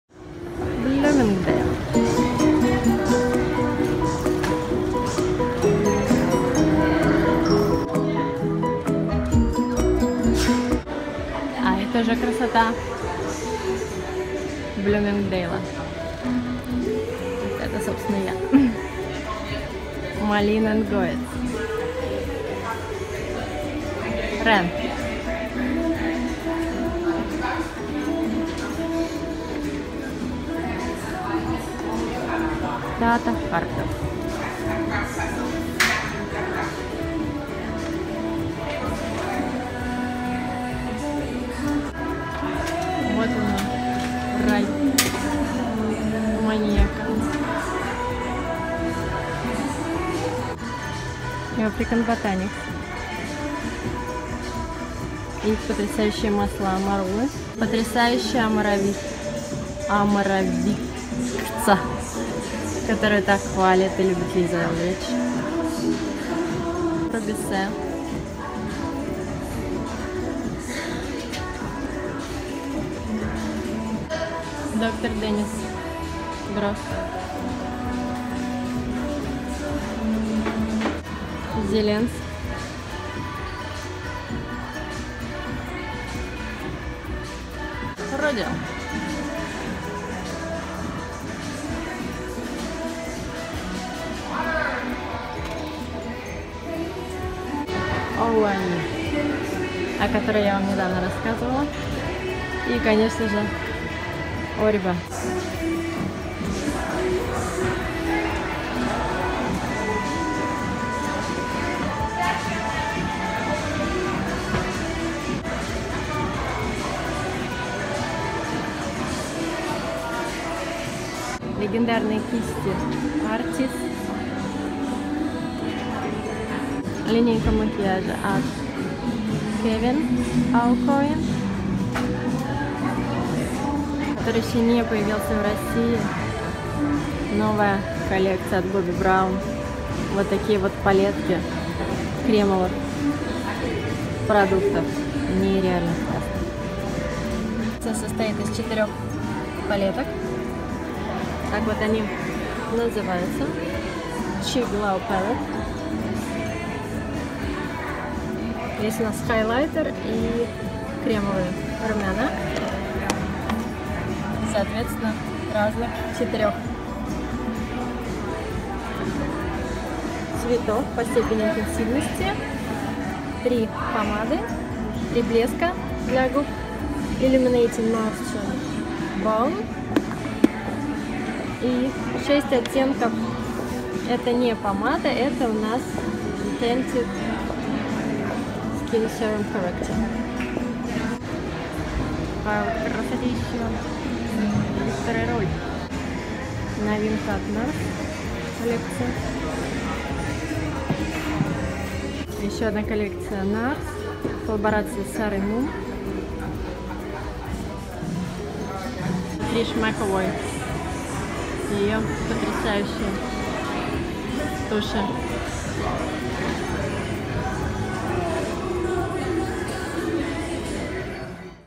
BLOOMINGDALE'S - эксклюзивный уход за кожей и волосами + новинки макияжа Bobbi Brown и Nars
Здравствуйте. В этом видео я устрою вам ещё одну небольшую экскурсию, на этот раз по Бьюти зоне магазина Bloomingdale's в Беверли Хиллз. Здесь представлены лучшие марки по уходу за кожей и волосами, такие как Tata Harper, Ren, Rodial, Eve Lom, Zelens, Oribe, Philip B, OUAI и др.  К тому же я покажу вам совершенно новые, совсем недавно поступившие в продажу, коллекции макияжа от марок Bobbi Brown и Nars. Надеюсь, данная информация будет вам полезна.